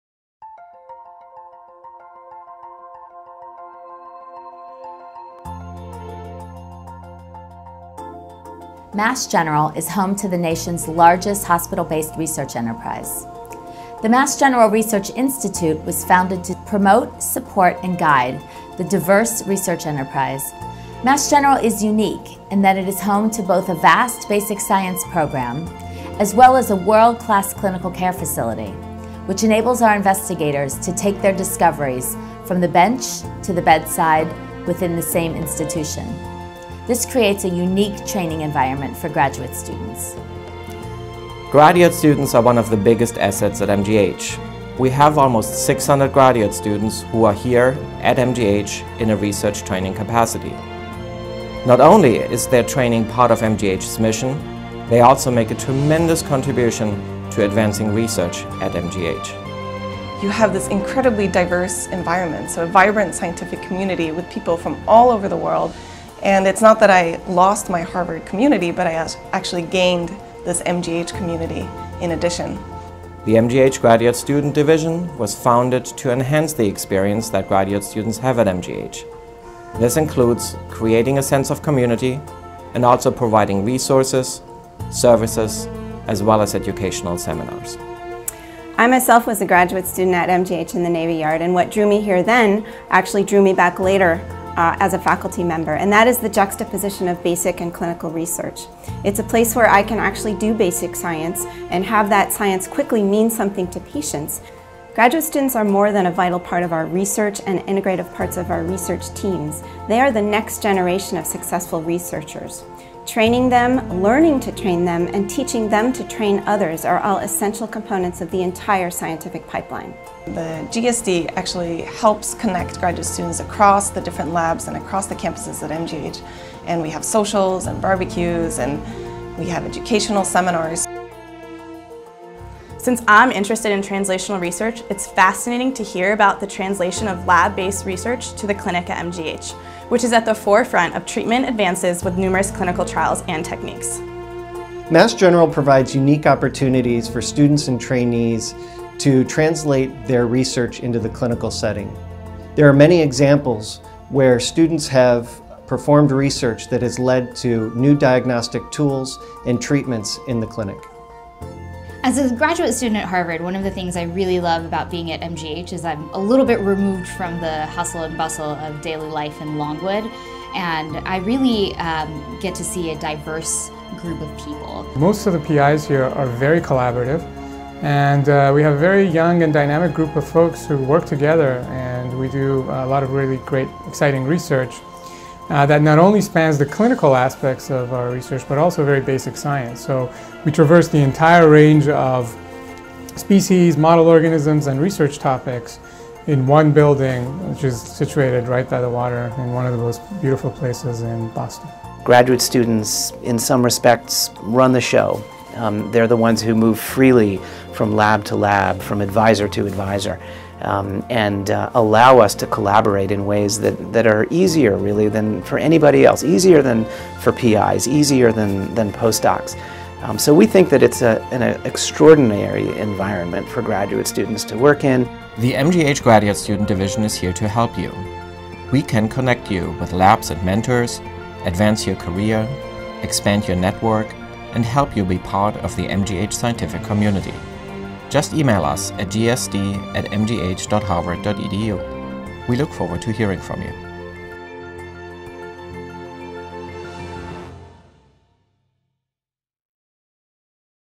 Massachusetts General Hospital Graduate Student Division
The Graduate Student Division (GSD) was created to foster a graduate student community and to serve the practical needs of graduate students from all academic institutions who are associated with basic and clinical research at MGH. The GSD areas of emphasis are: serve basic and academic needs of graduate students, provide programs, services, and resources, create a sense of community, enhance the overall experience of students affiliated with MGH, strengthen relationships with area graduate schools, and attract more graduate students to MGH. Info: